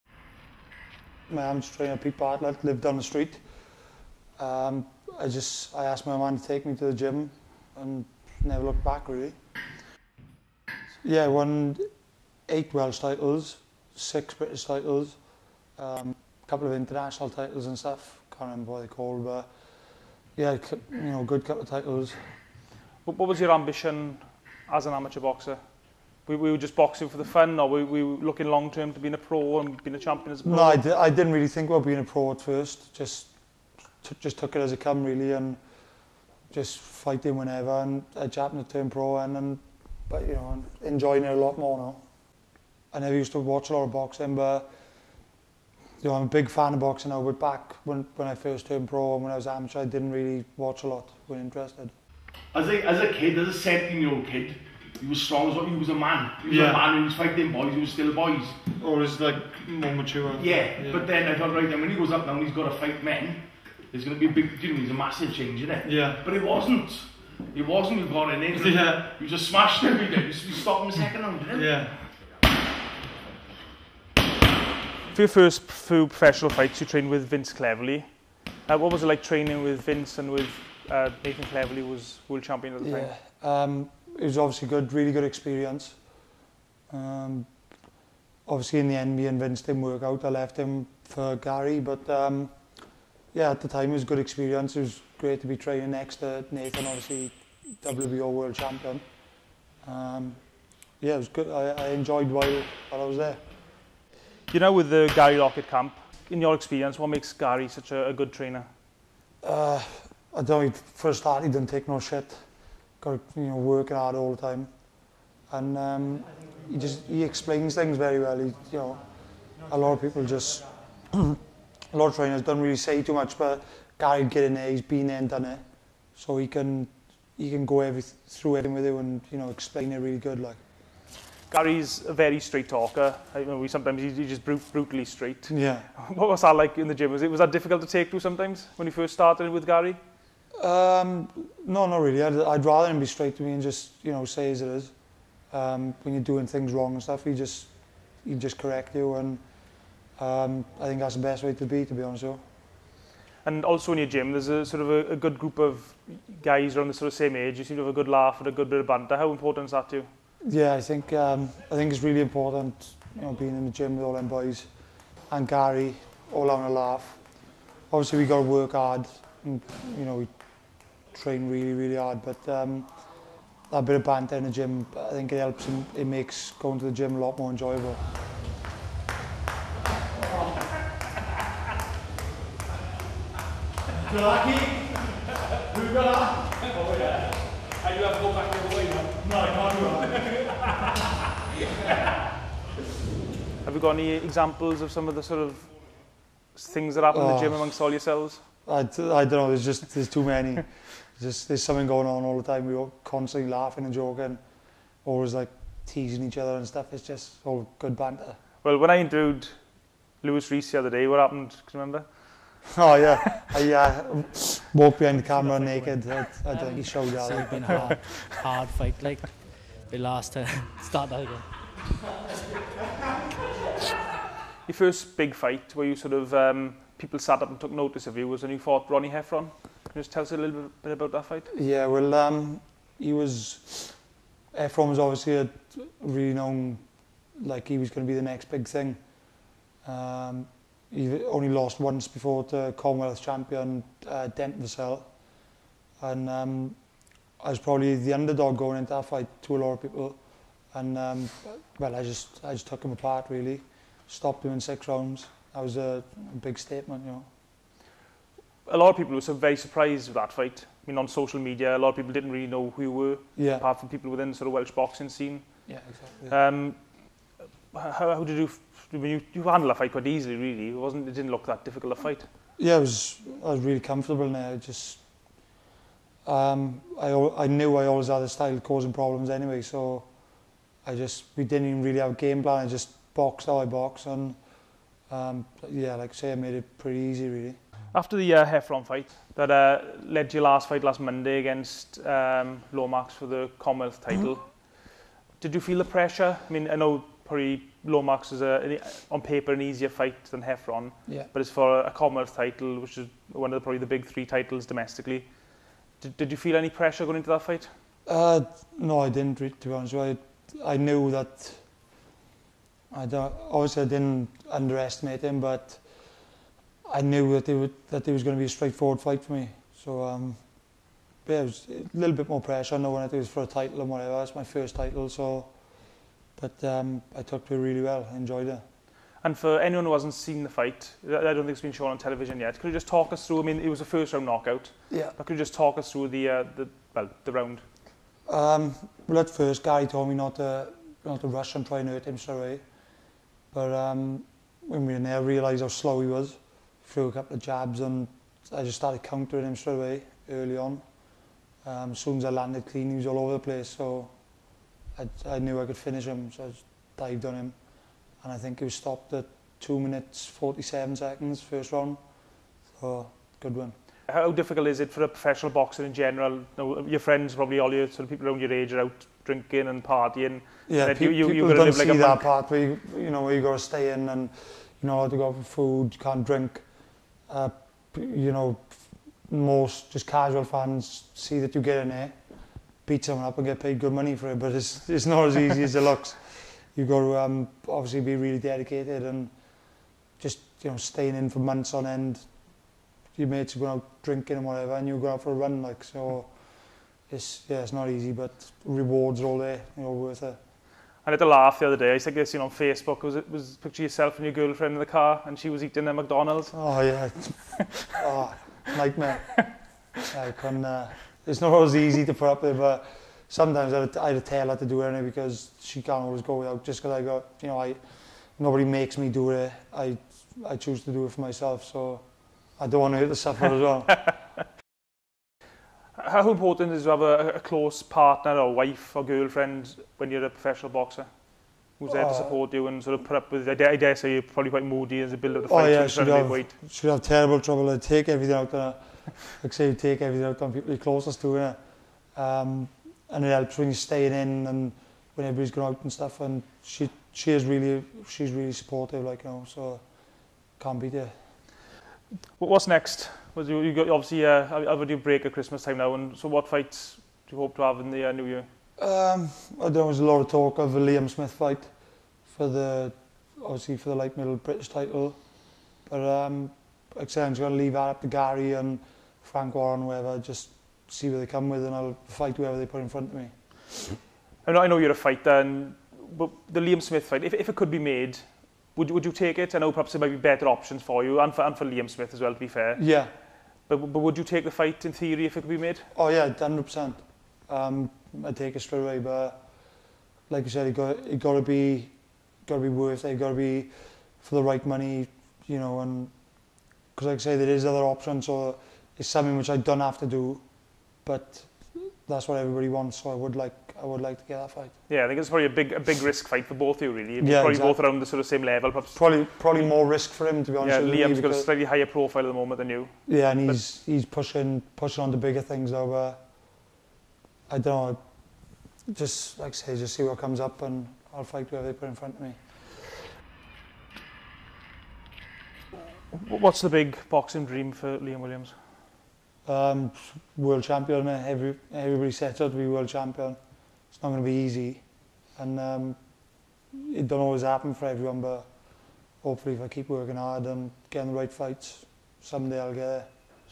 Young Welsh boxer LIAM WILLIAMS sets his sights on titles
Commonwealth boxing champion Liam Williams talks to @CiaranGibbons1 about boxing career to date.